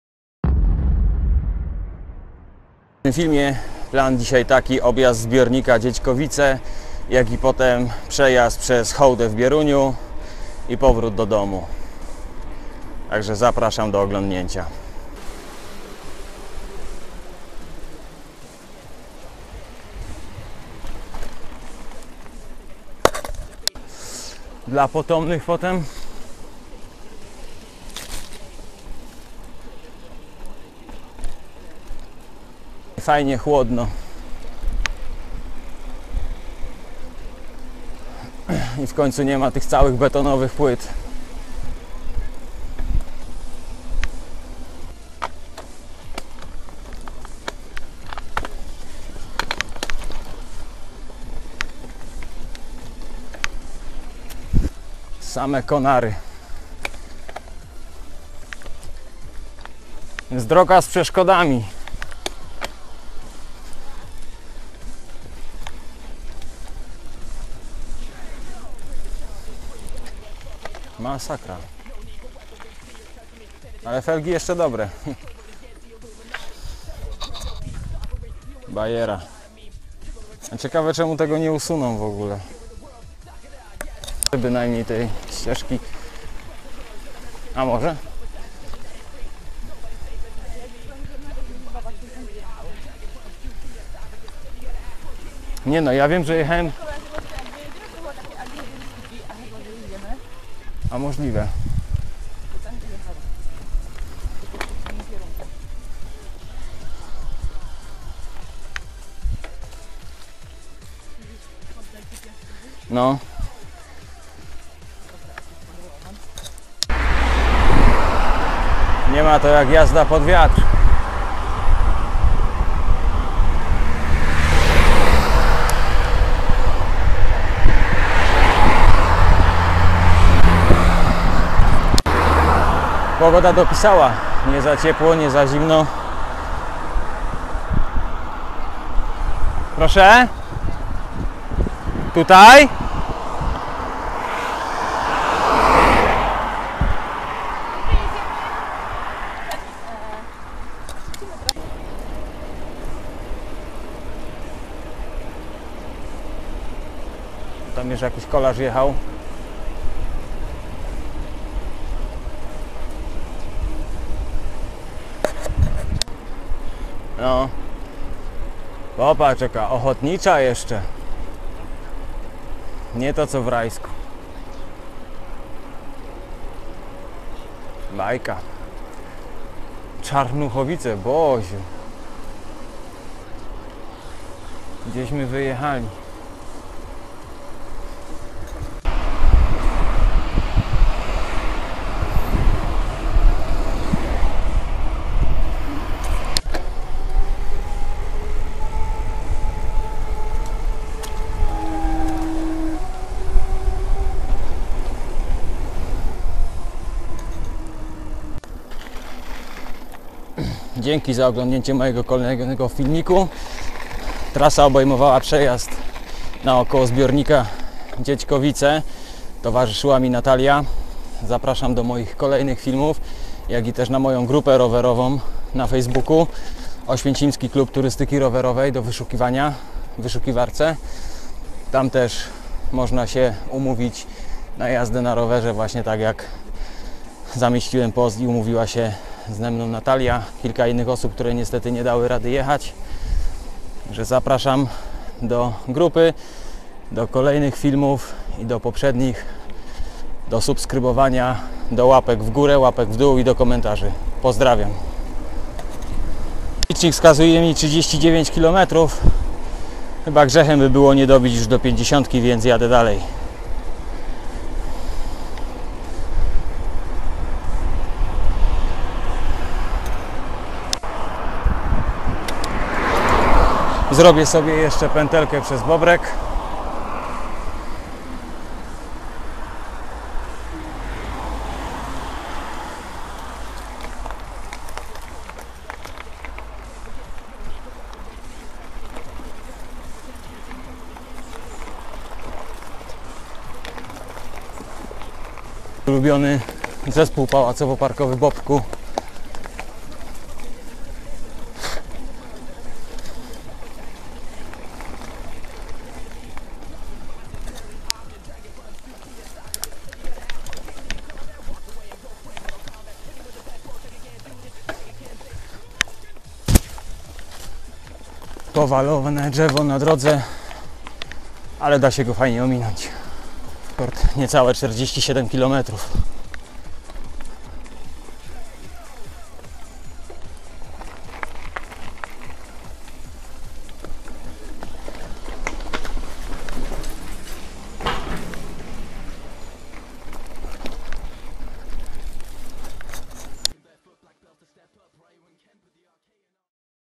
Trip na około zbiornika Dziećkowice 19.08.2018 By Xiaomi Yi
Dzisiejszy trip na około zbiornika Dziećkowice ze znajomą
gdzie mozna sie zgadać na jakąś przejażdżke tak jak ja z Natalią